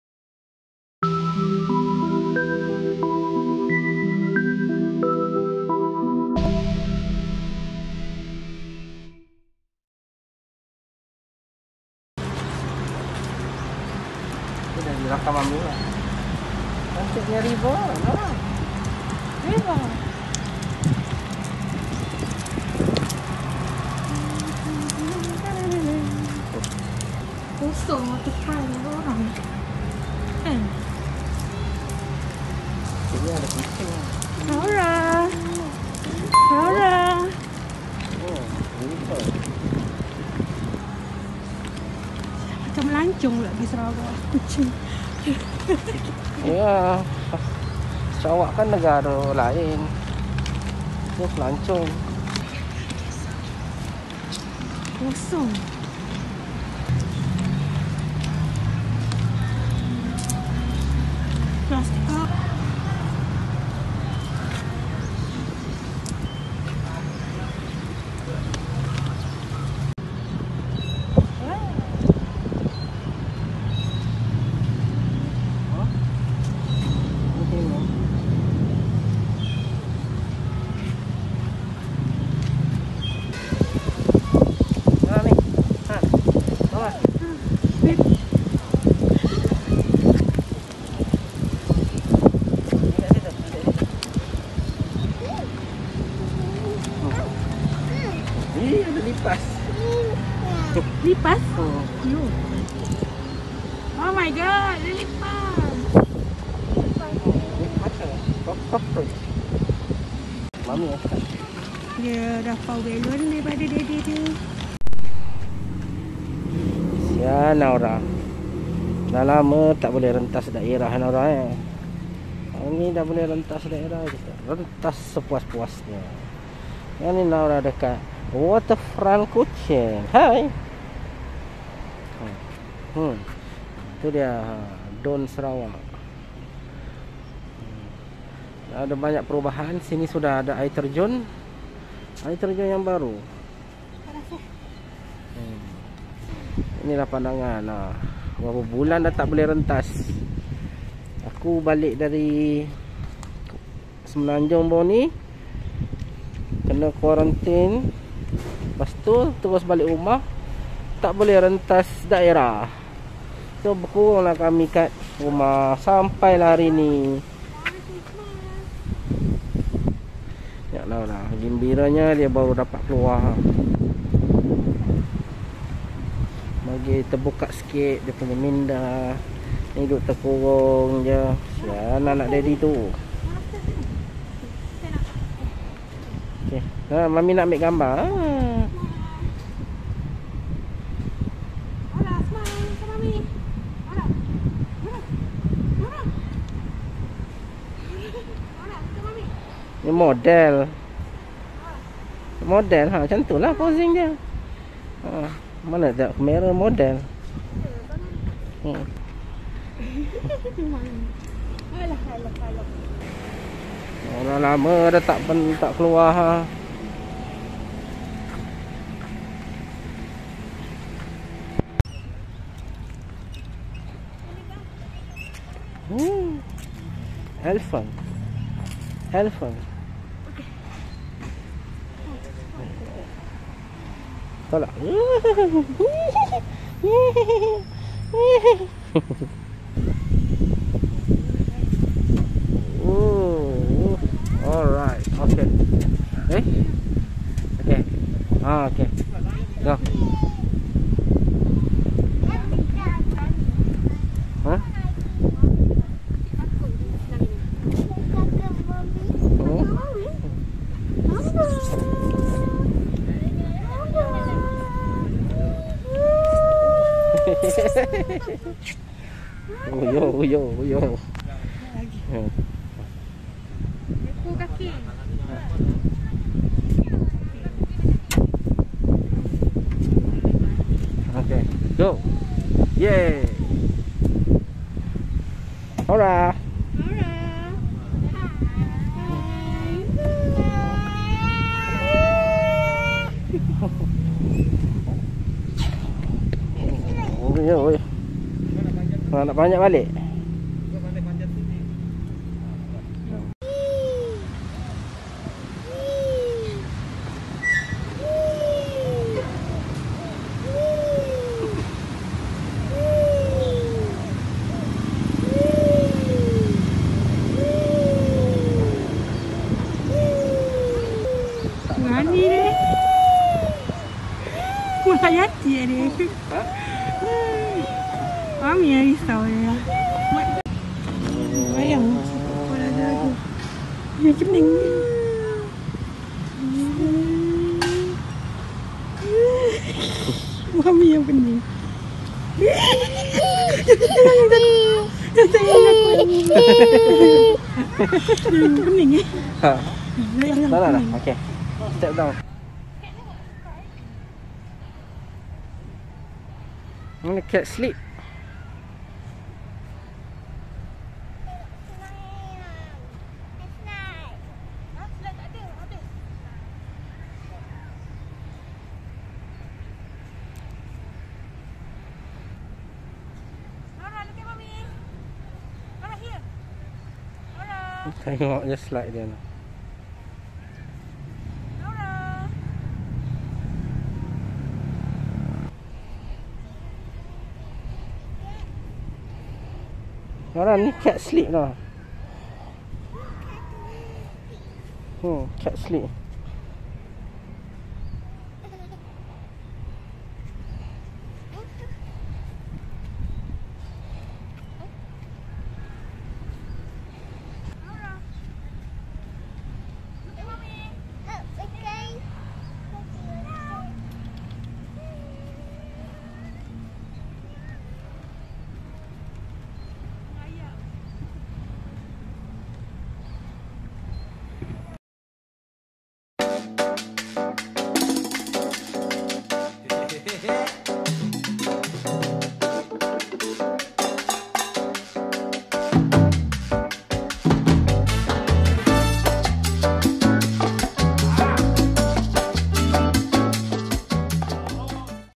VLOG: Naura di Waterfront Kuching | Setelah Sekian Lama Tidak Dapat Rentas Daerah
Setelah sekian lama, akhirnya dapat rentas daerah jua. Yang paling gembira adalah Naura kerana sudah tidak dapat rentas daerah sekian lama.

Aku memang selalu guna muzik-muzik kat sini sebab memang best muzik-muzik dia. Percuma sebulan.